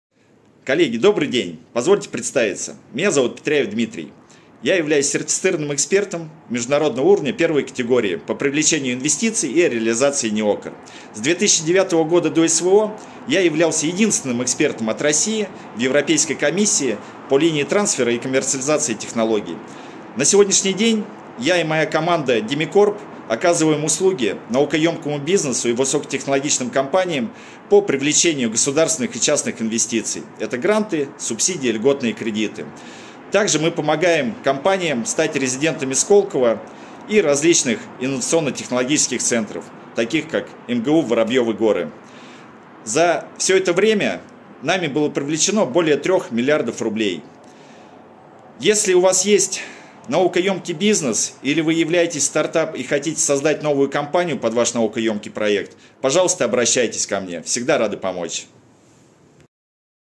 Видео резюме основателя ДИМИКОРП
Я основатель компании ДИМИКОРП, моя команда более 16 лет успешно привлекает государственные и частные инвестиции в наукоемкие компании, помогаем компаниям получить статус резидента Сколково и других ИНТЦ.
➡️ Более 40 Заказчиков получили статус резидента Сколково и других ИНТЦ.
➡️ Было привлечено более 4 млрд, рублей!